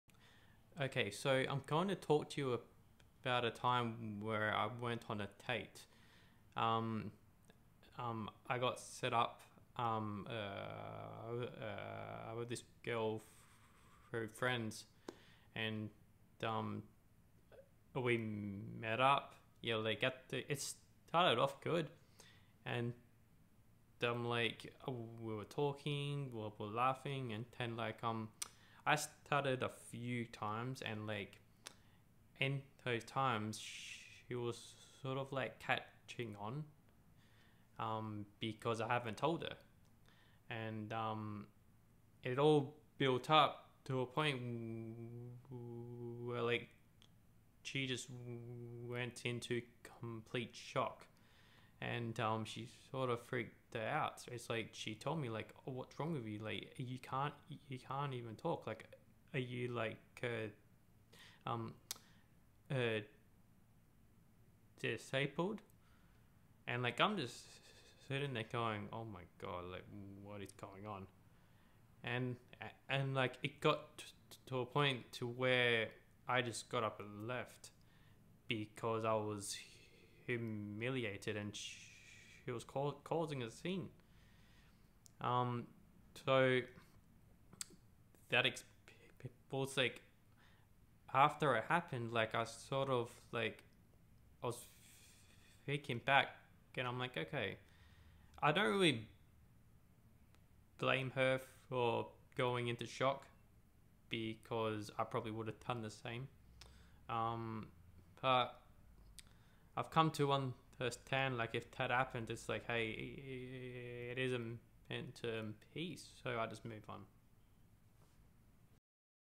My Worst Stuttering Date Story...
This is about my worst date experience, this happened because I was stuttering in front of her and it caught her off-guard and she freaked out!...